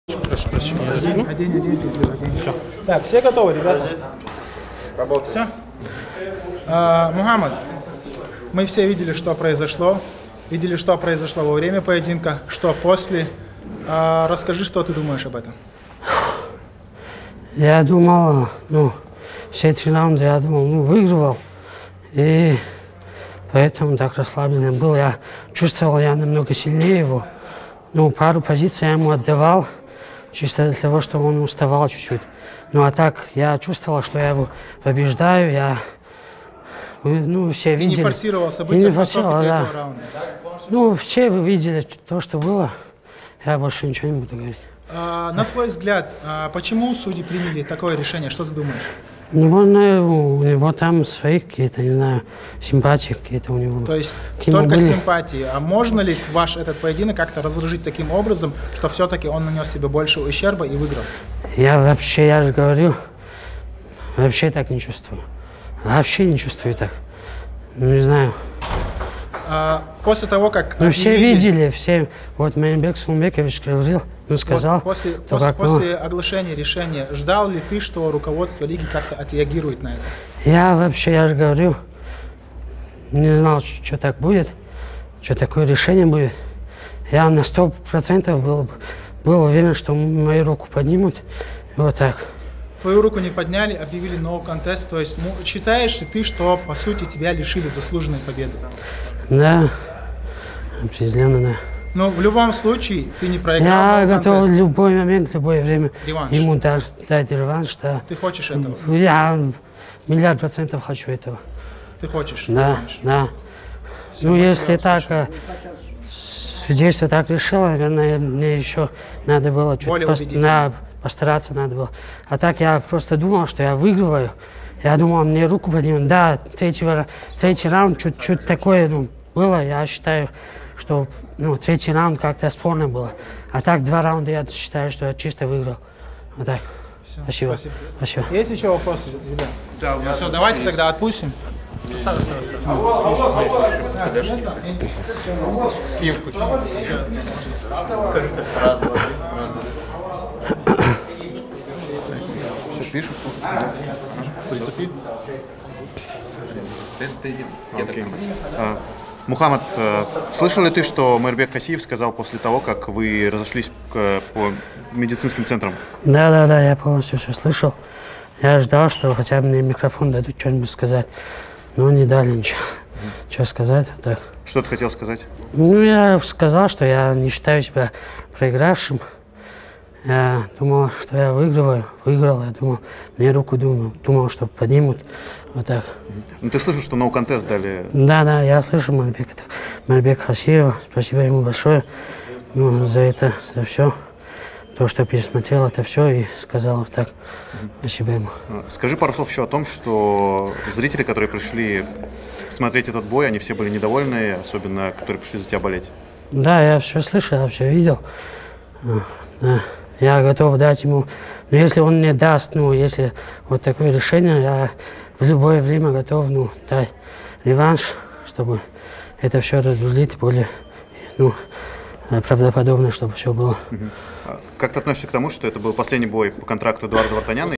Мухаммад Коков - "я был уверен в своей победе" / Интервью после боя
После окончания боя между Эдуардом Вартаняном и Мухаммадом Коковым президент ACA Маирбек Хасиев выступил с критикой судей и высказался о признании статуса боя несостоявшимся